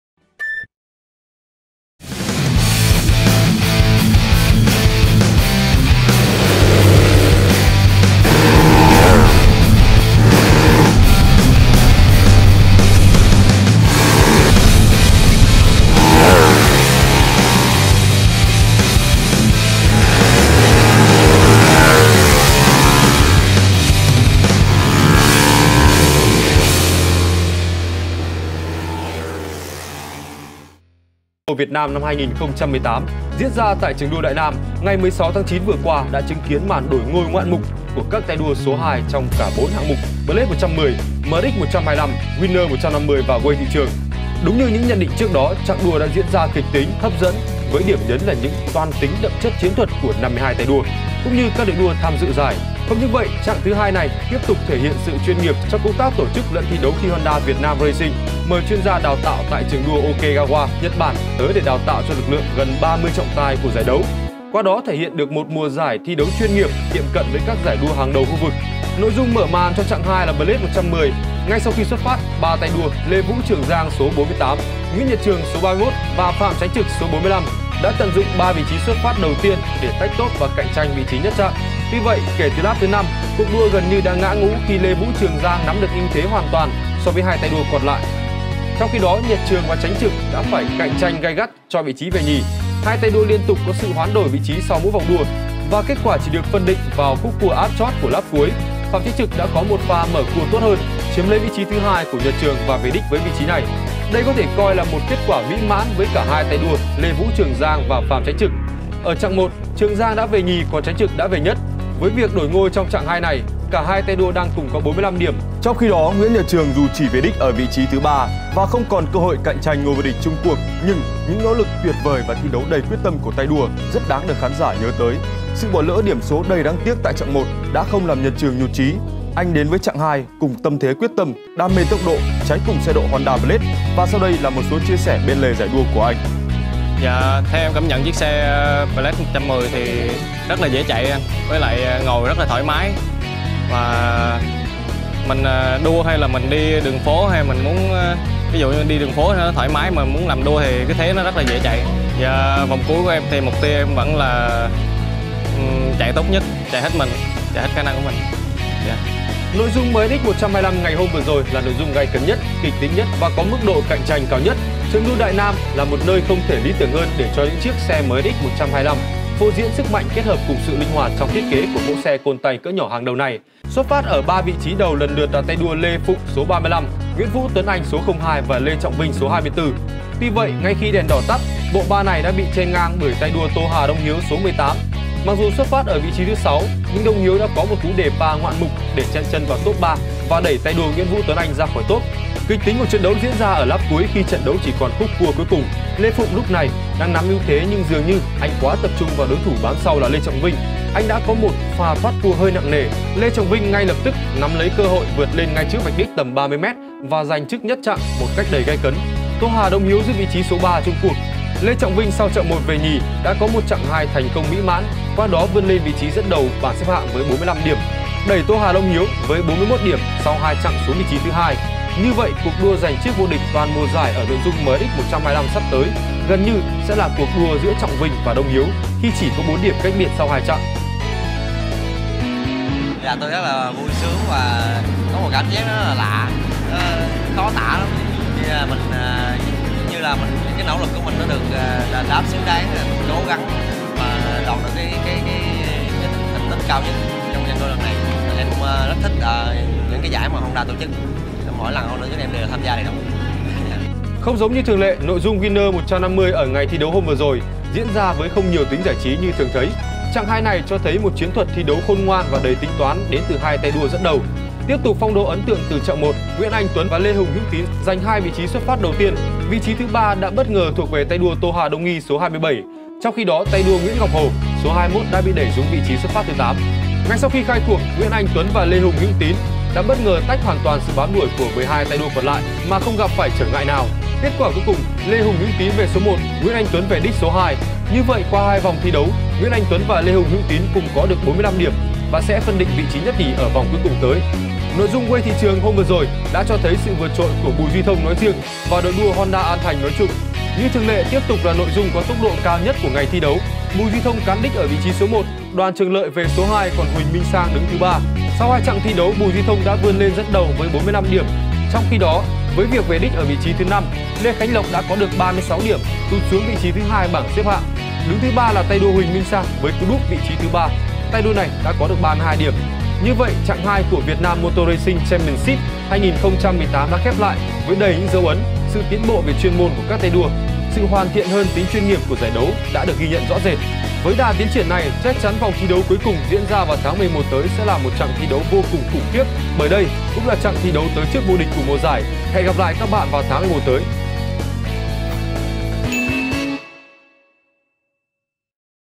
Chặng 2 Giải đua xe Mô tô Việt Nam 2018: Đổi ngôi ngoạn mục |AUTODAILY.VN
Chặng 2 Giải đua xe Mô tô Việt Nam 2018 diễn ra tại trường đua Đại Nam cuối tuần qua đã chứng kiến màn đổi ngôi ngoạn mục của các tay đua số 2 trong cả bốn hạng mục: Blade 110, MSX 125, Winner 150 và Wave thị trường.

Đúng như những nhận định trước đó, chặng đua đã diễn ra kịch tính, hấp dẫn với điểm nhấn là những toan tính đậm chất chiến thuật của 52 tay đua cũng như các đội đua tham dự giải. Không những vây, chặng thứ 2 này tiếp tục thể hiện sự chuyên nghiệp trong công tác tổ chức lẫn thi đấu khi Honda Việt Nam Racing mời chuyên gia đào tạo tại trường đua Okegawa (Nhật Bản) tới để đào tạo cho lực lượng gần 30 trọng tài của giải đấu, qua đó thể hiện được một giải đấu chuyên nghiệp tiệm cận với các giải đua hàng đầu khu vực.

Nội dung mở màn cho chặng 2 là Blade 110. Ngay sau khi xuất phát, 3 tay đua Lê Vũ Trường Giang (số 48), Nguyễn Nhật Trường (số 31) và Phạm Chánh Trực (số 45) đã tận dụng 3 vị trí xuất phát đầu tiên để tách top và cạnh tranh vị trí nhất chặng. Tuy vậy, kể từ lap thứ 5, cuộc đua gần như đã ngã ngũ khi Lê Vũ Trường Giang nắm được ưu thế hoàn toàn so với 2 tay đua còn lại. Trong khi đó Nhật Trường và Chánh Trực đã phải cạnh tranh gay gắt cho vị trí về nhì, 2 tay đua liên tục có sự hoán đổi vị trí sau mỗi vòng đua. Và kết quả chỉ được phân định vào khúc cua áp chót của lap cuối, Phạm Chánh Trực đã có một pha mở cua tốt hơn, chiếm lấy vị trí thứ 2 của Nhật Trường và về đích với vị trí này.